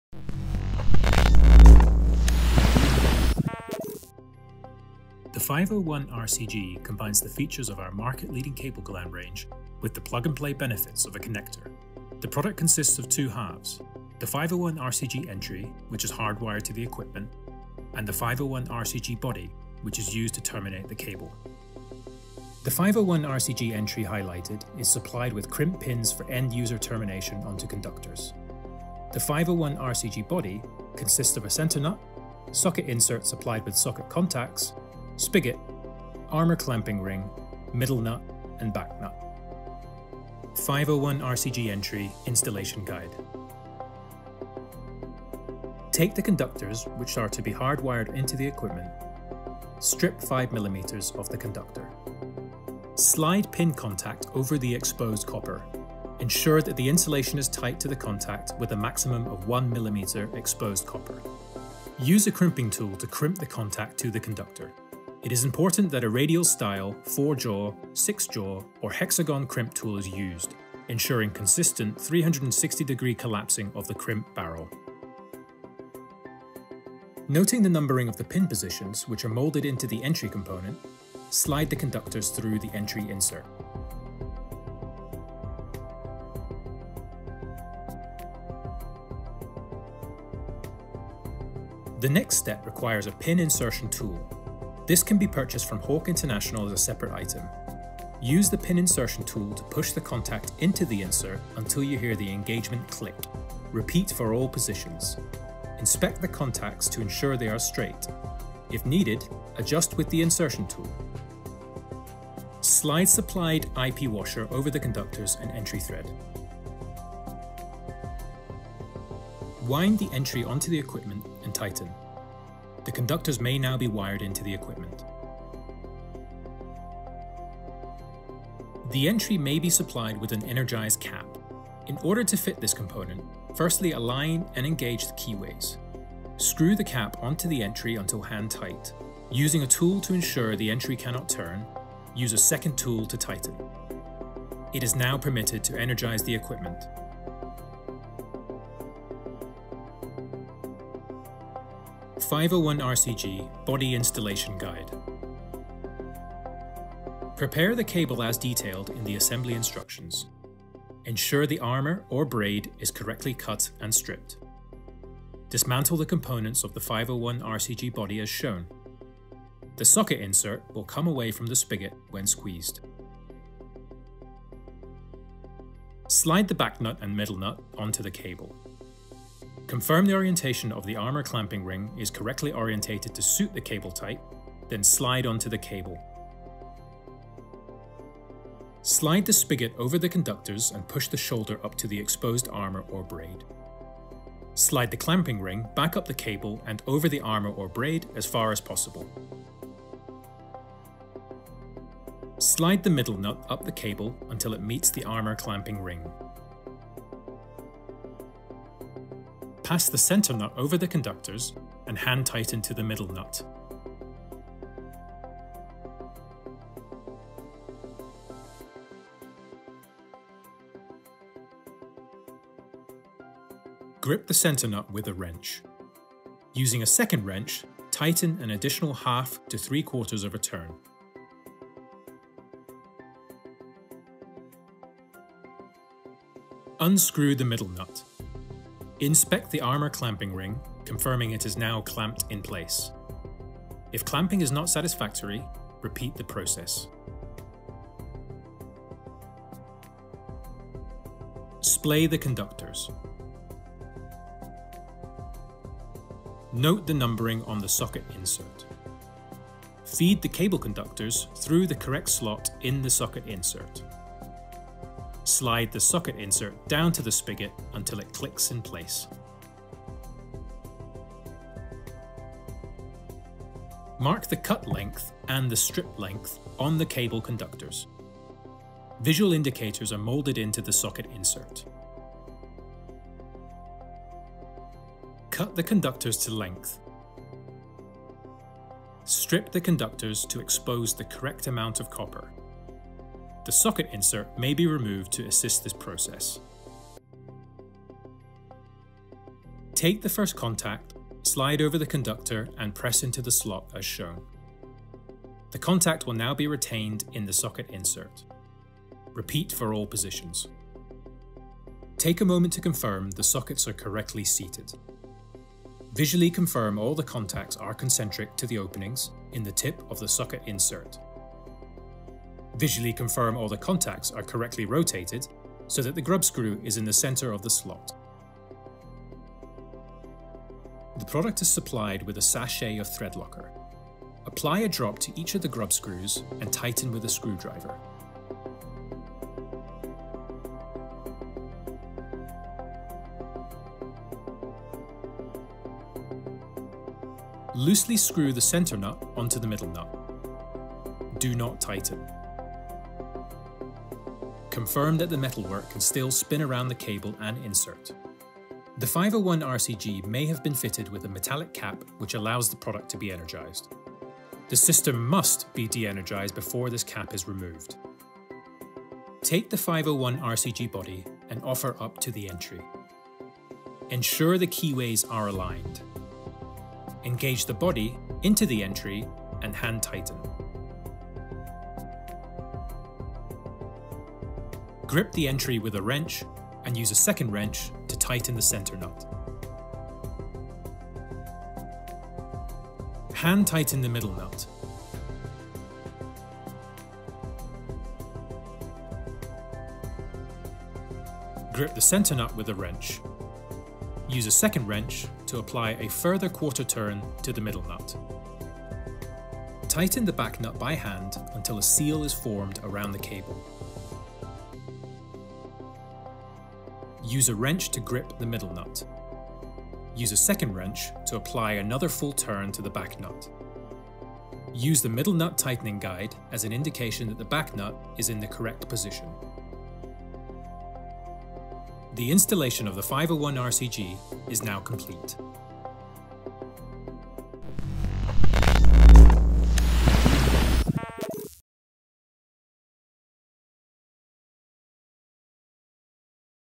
501/RCG | Cable Gland Installation Guide | Hawke International
Watch us demonstrate how to correctly install the 501/RCG Rapid Connection Gland